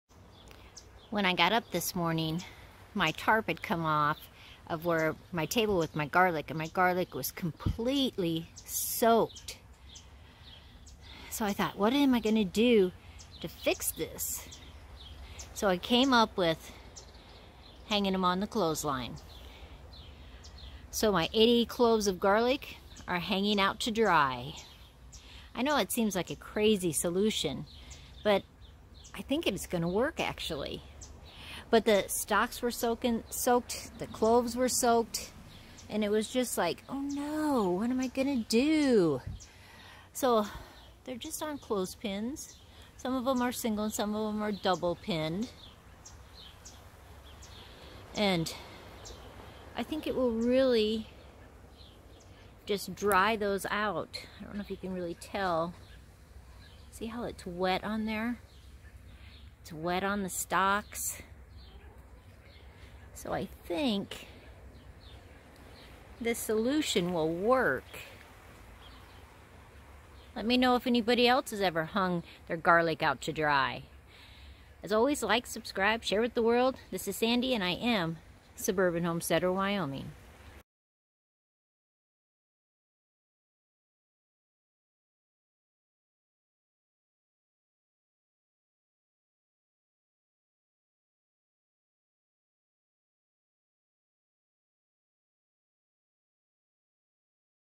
Oh No! They got all WET!
When I got up this morning, my tarp over my garlic had moved and all the garlic was soaking wet.  What was I going to do? My solution was to hang them out to dry. I hung all 80 bulbs on my clothes line. I think it should work. We will have temperatures in the 90's today.